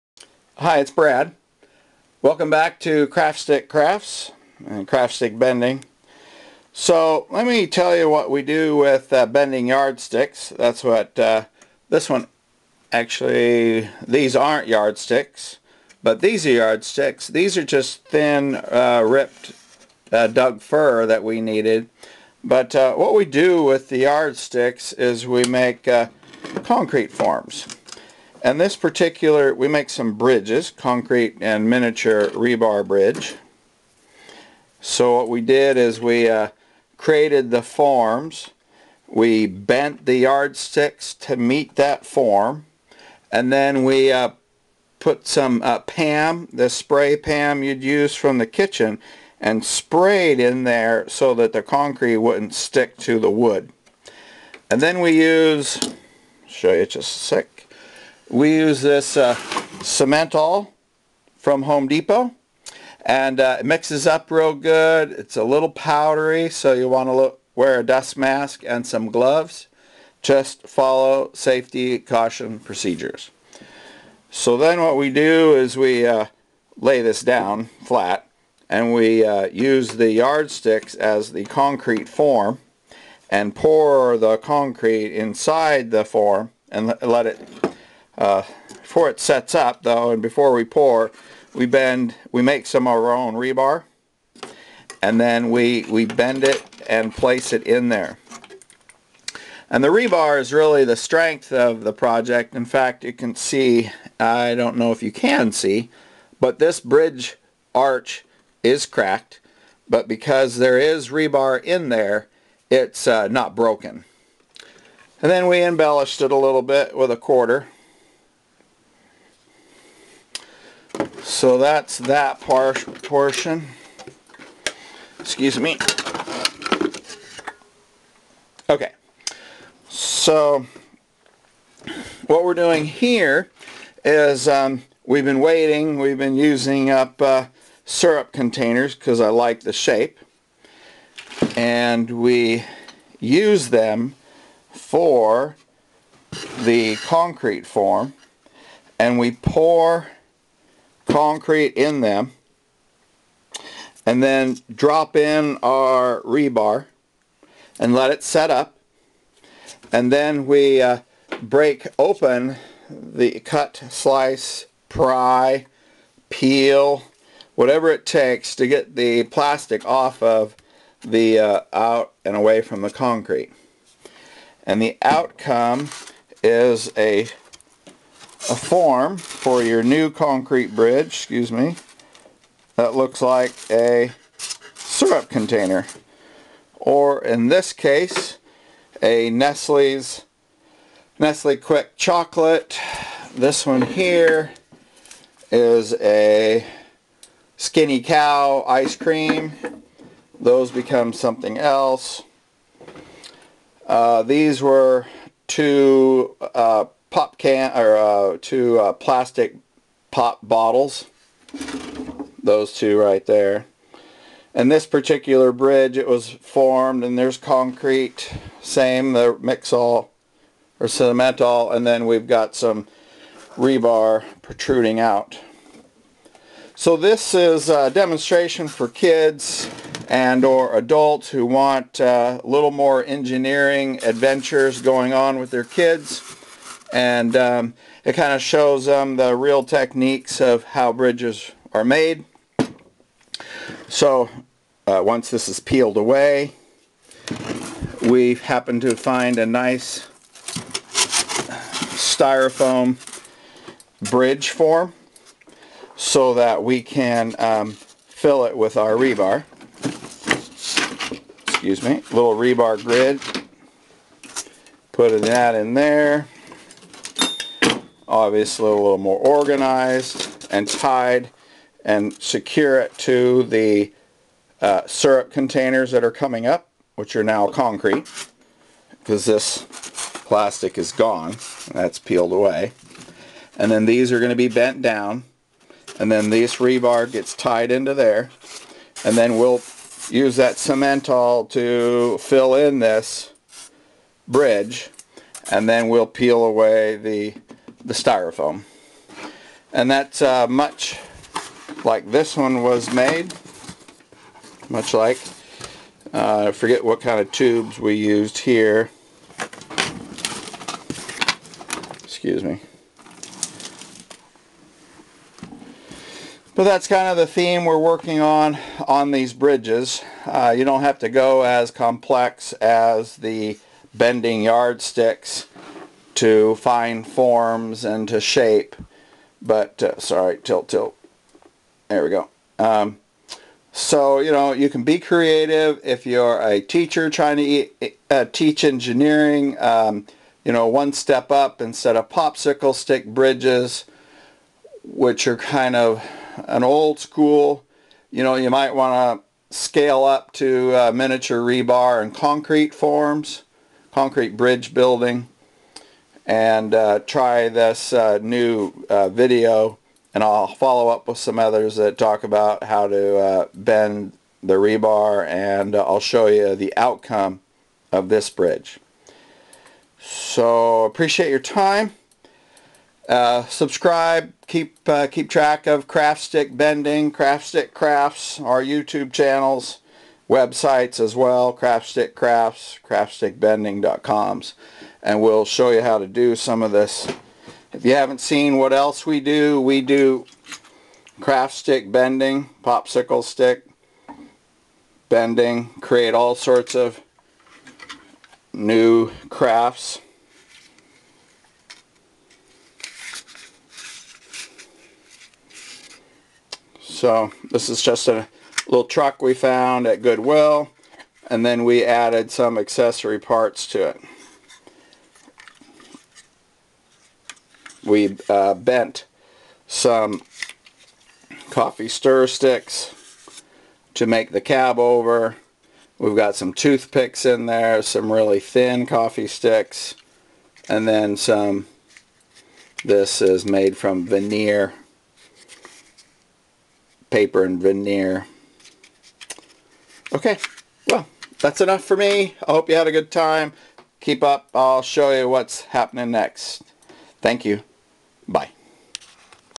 Craft Stick Crafts Video 1 Concrete and rebar School Bridge Engineering Projects
Looking to the future, I'm working on what is next to inspire young minds. I make miniature rebar to be used in small scale concrete bridges. I've been playing with this idea for a while, making it easy to do from items around the house. Teachers could teach this in engineering classes like they teach Popsicle stick bridge building. The nation needs engineers; this is my way to make a difference. One man can make a difference.